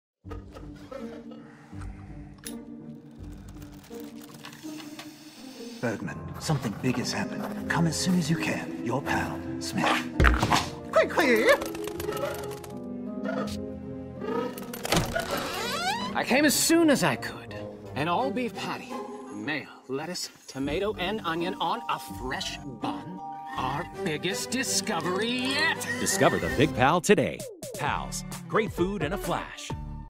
Pal's - Big Discovery
Agency  : Creative Energy
Campaign : Big Discovery
Advertiser : Pal's
Brand  : Pal's
Length  : 30 Seconds
Posted   : Apr 24, 2024

In this Movie Ad we’ll walk you through:
"Big Discovery," the ad cleverly portrays a moment of urgency where a character rushes to deliver significant news.

TIMESTAMPS :
00:00 Pal's - Big Discovery
Plot :
Expecting significant news, he arrives to find a colleague eagerly awaiting him, only to be presented with Pal's new "Big Pal" burger, emphasizing its size and fresh ingredients. Bergman's initial confusion turns into delight as he indulges in the delicious burger, highlighting the unexpected joy of discovering Pal's new menu offering.